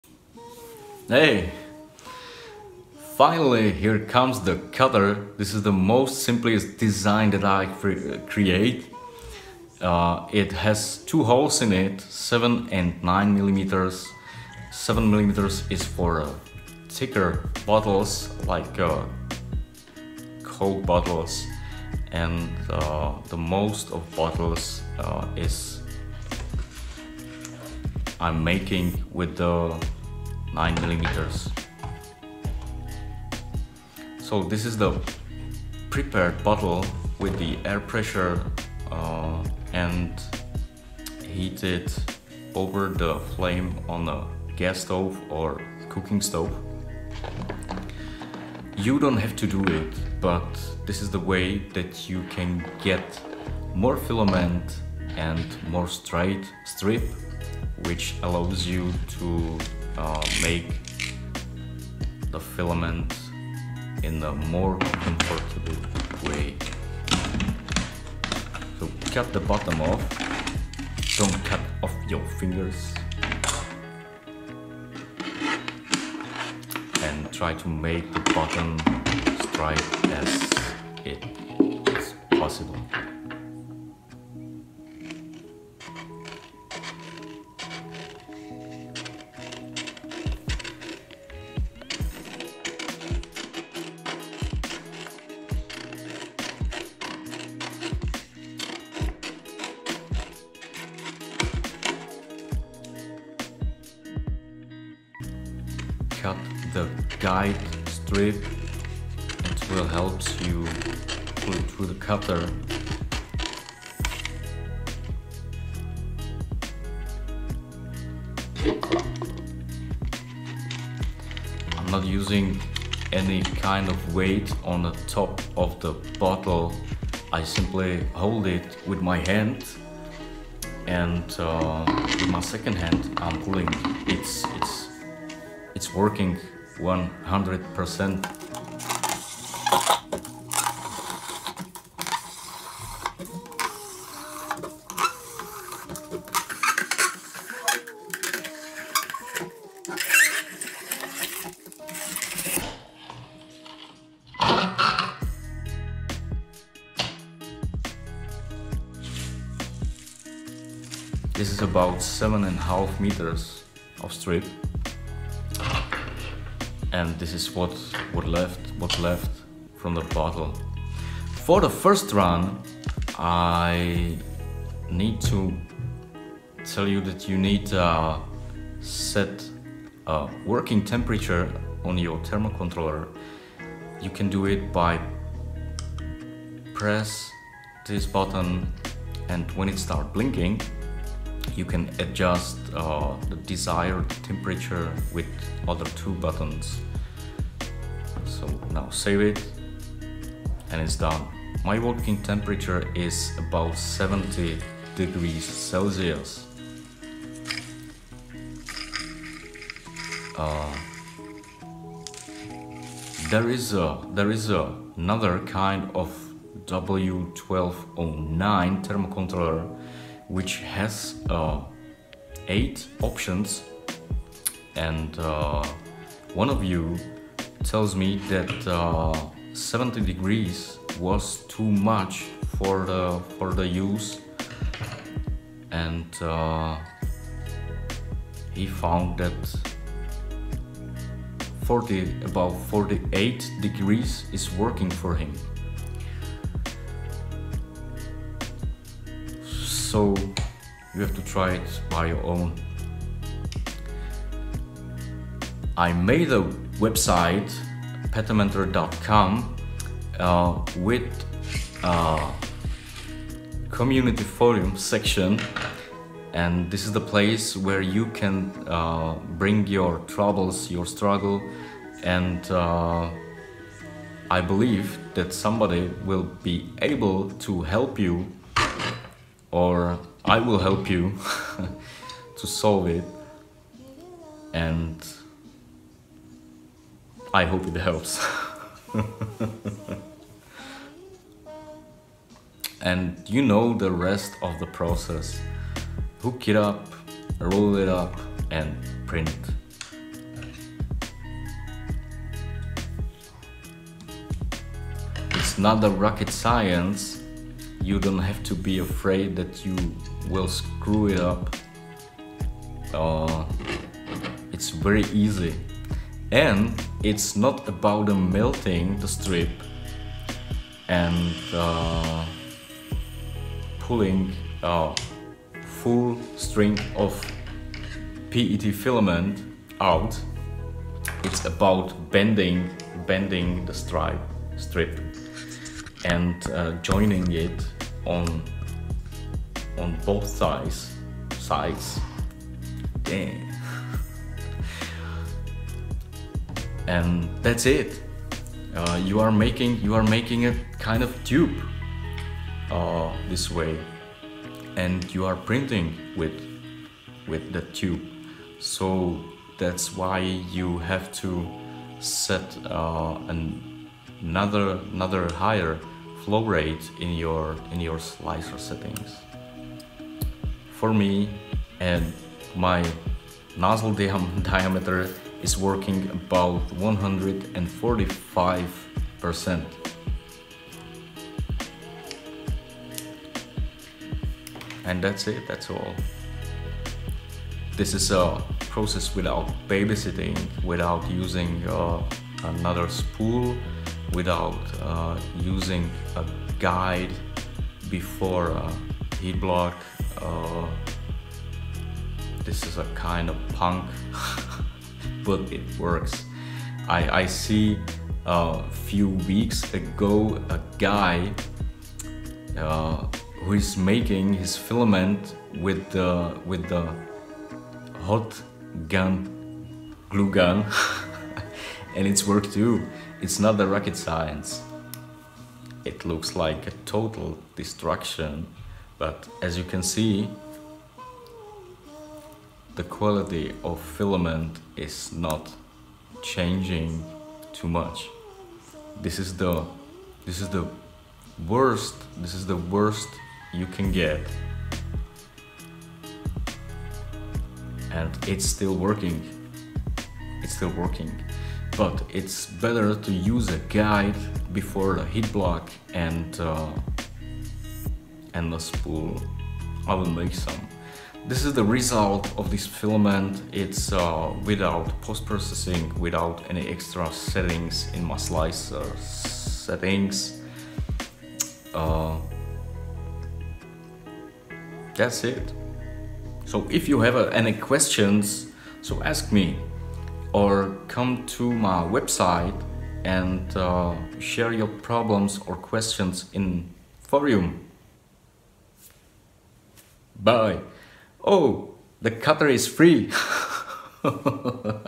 Petamentor bottle cutter and first run
Just wanna say Hi and THANK YOU FOR YOUR SUPPORT WITH LIKES! ❤️
DIY PET filament from Bottles with DIY Petamentor machine 💪